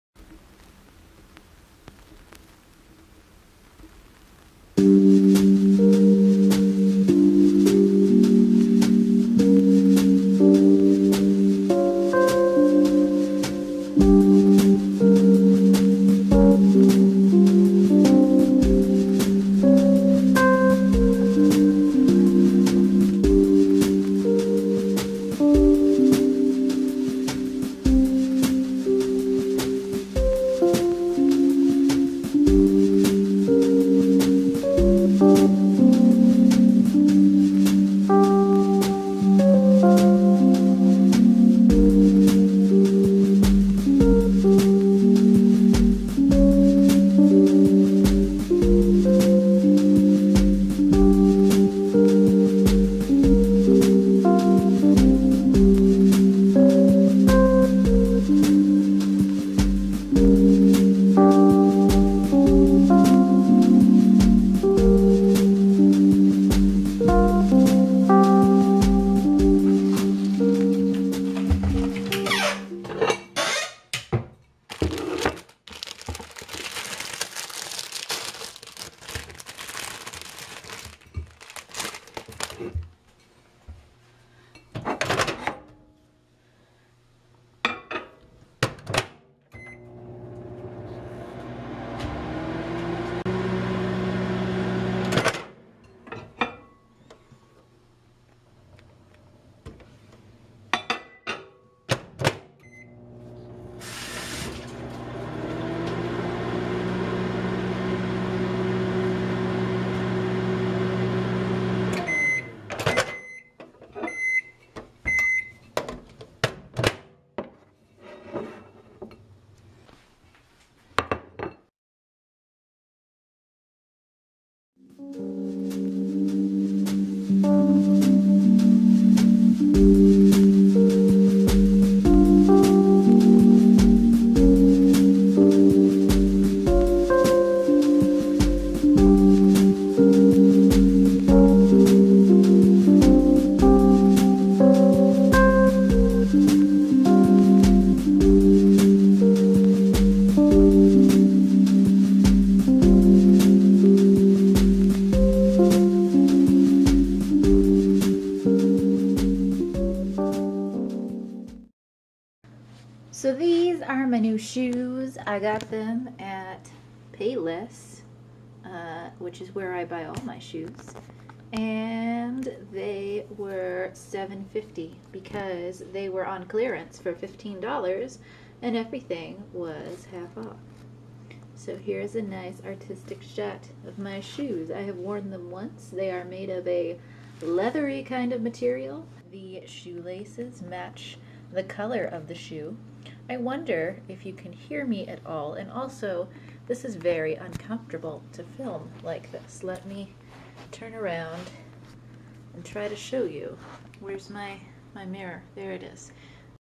library book sale vlog (feat. pretzel and cheese)
Hello! I wanted to try a vlog even though that's not my normal thing, so here's video from a few Saturdays ago when our library had its big used book sale. We have these every so often and believe me, they're SUPER crowded--I really had to be stealthy to film (just a tiny portion of!) the shelves in order not to have random people in my video.

This is just an experiment because I really love watching vlogs lately and I used to make them (when I had another channel in what was practically another lifetime). Let me know what y'all think? (I know it's short!)

Also if you watch to the end you can listen to me ramble about my shoes.